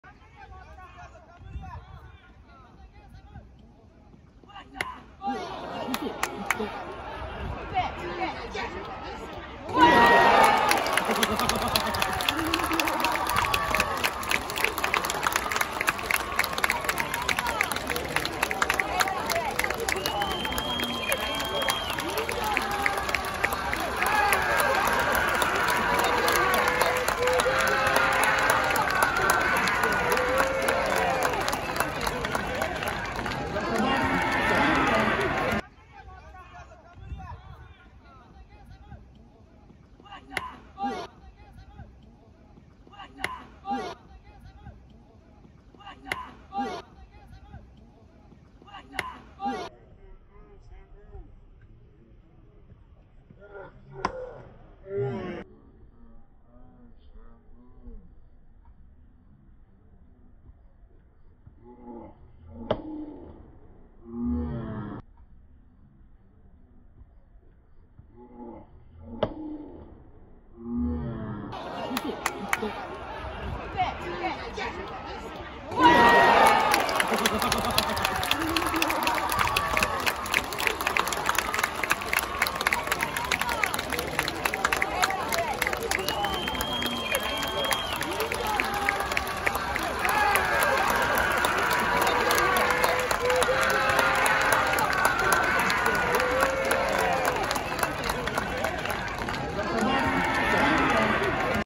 江戸川区立上一色中 4番 福原勘太 ホームラン！
江戸川区立上一色中学　第３９回全日本少年軟式野球大会　シートノック
youtu.be/a-qtSbkZzkU